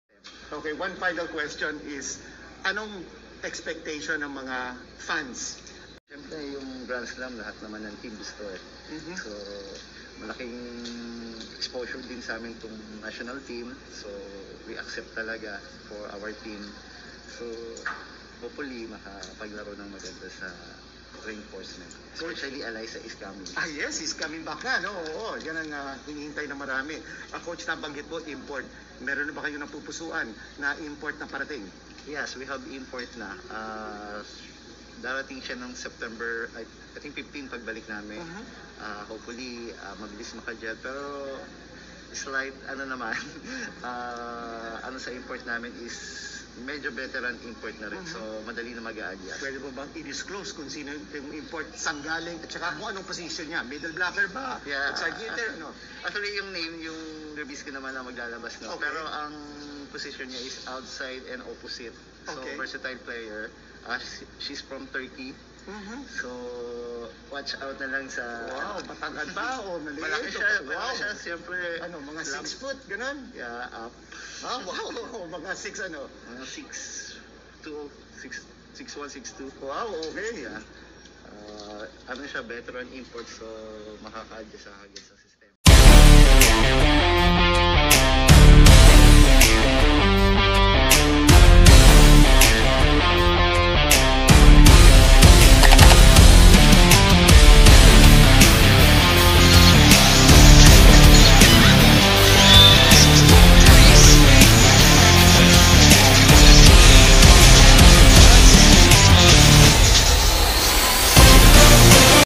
Turkey ang import ng creamline cool smasher ccs this coming pvl reinforce conference 2022
FORDA GRANDSLAM ANG CCS!!! 🔥

So Creamline Cool Smasher's Import will be a TURKISH WING SPIKER???

Wow, CCS is Serious!! We all know that Turkey is one of the best when it comes to volleyball! There are so many great volleyball players there!!!

Grandslam Naba this year??? 🤭

Sana si KARAKURT!!! Kahit opposite position niya pwede naman siyang mag adjust!! Para KARAKURT and TOTS CARLOS (KARAKURT ng Pinas) ang bobomba!! TWO POWERFUL PINK HAIR SPIKERS!!! 🔥

May nakita akong post, sabi daw ni Coach Sherwin and ang sabi sa Sept. 15 daw darating ang import nila aftrer Grand Prix!! (nag refresh si fb kaya hindi ko na mahanap ang post huhu)